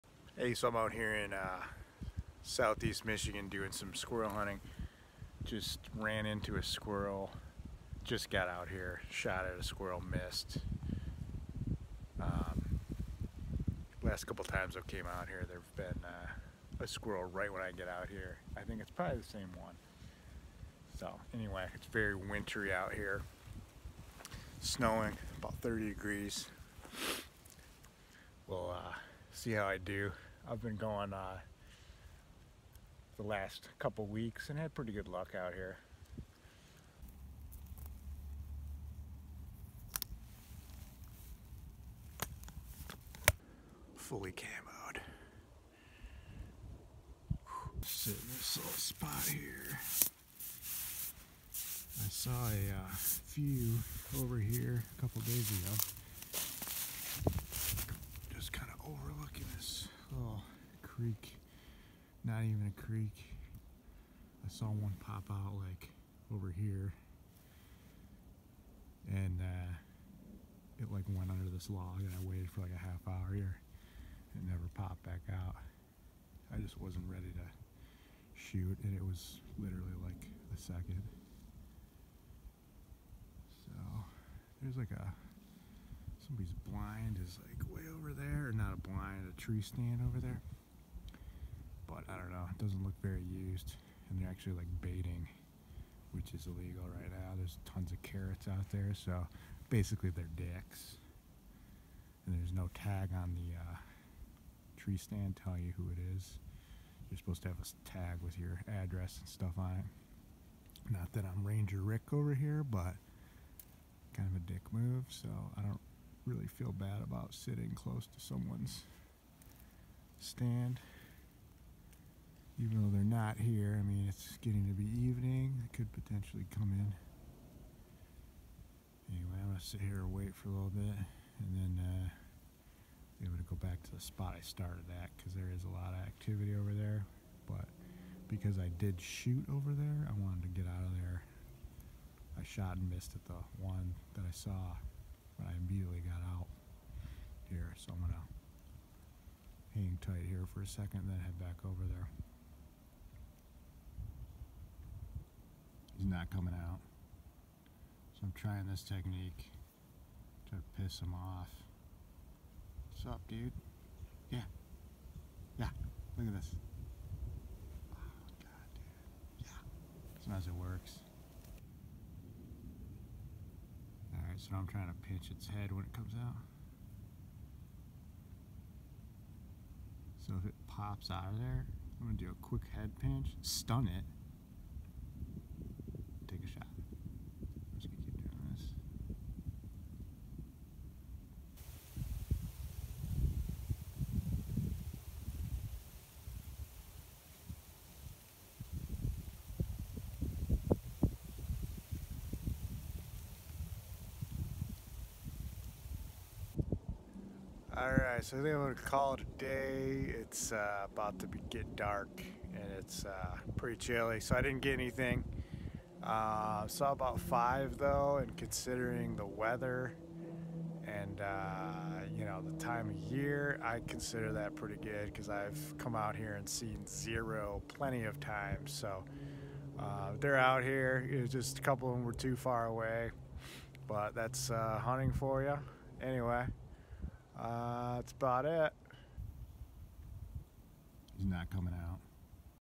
Winter Squirrel Hunting - Tips and Tricks* (*No legitimate tips and/or tricks are in this video)
Join me on this winter adventure. I share some of my tips and tricks for winter squirrel hunting, including the middle finger technique and the patented long distance finger pinch / head crush. Enjoy.